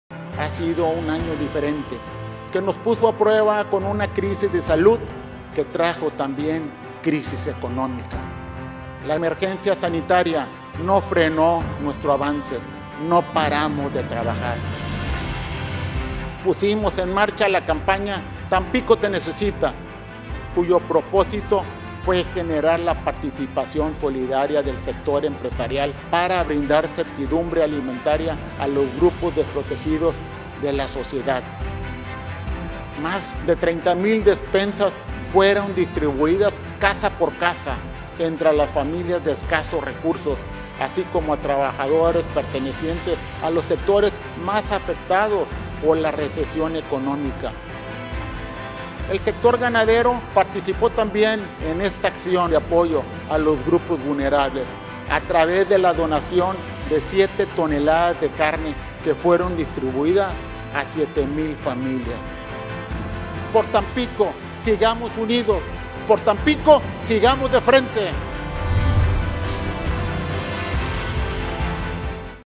En Tampico el COVID-19 no frenó el avance y más de 30 mil despensas fueron distribuidas a familias
"La emergencia sanitaria no frenó nuestro avance. Más de 30 mil despensas fueron distribuidas casa por casa entre las familias de escasos recursos, así como a trabajadores pertenecientes a los sectores más afectados por la recesión económica."